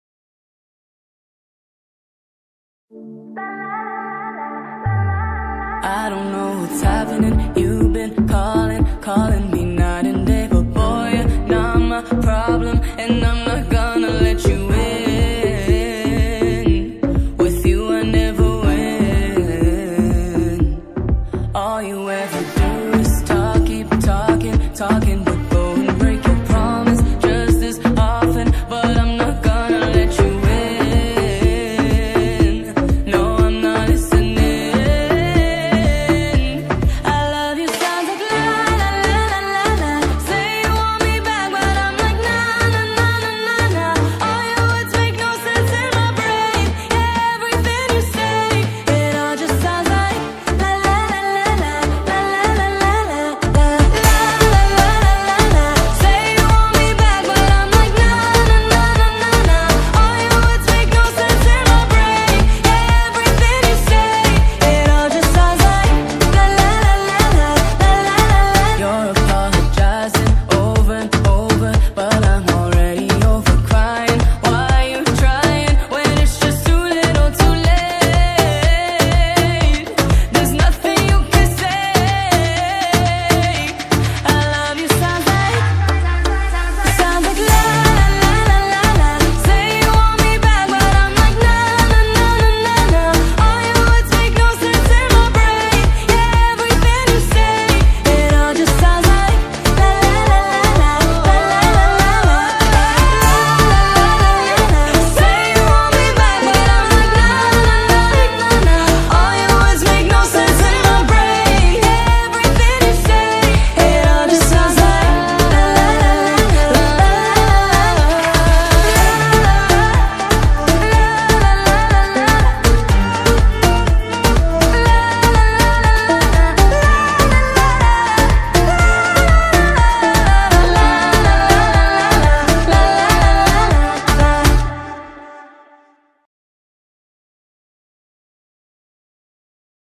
La La La Line Dance | Improver | @Esmeraldamm (NL)
La La La Line Dance

32/2 - Improver
1xTag (4C after wall 5)
Intro: 8C

Choreo:
Esmeralda van de Pol (NL) - May 2024

Demo: ArNew LD